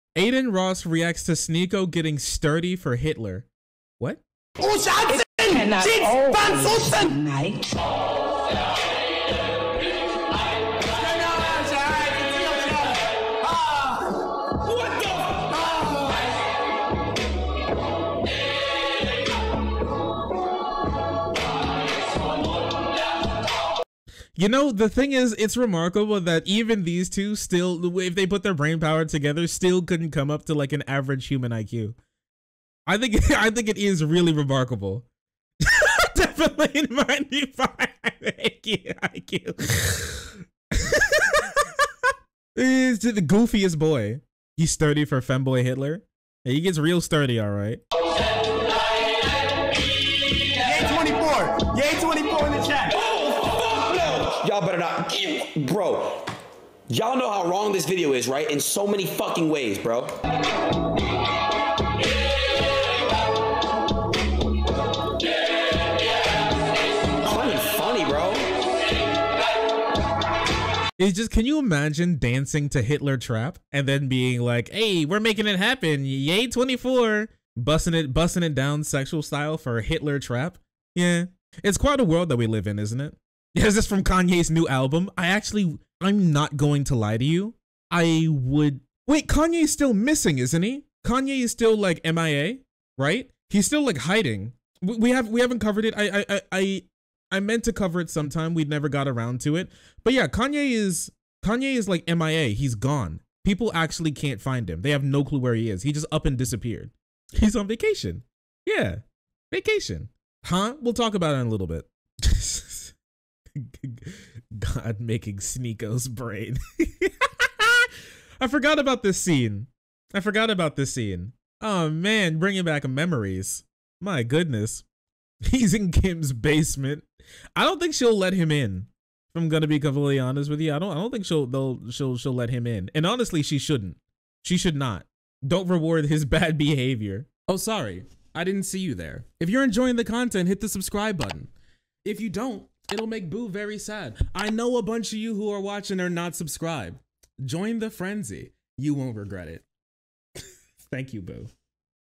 Sneako Does Bizarre Dance for Kanye
Sneako made a vid where he danced in support of Kanye West for president.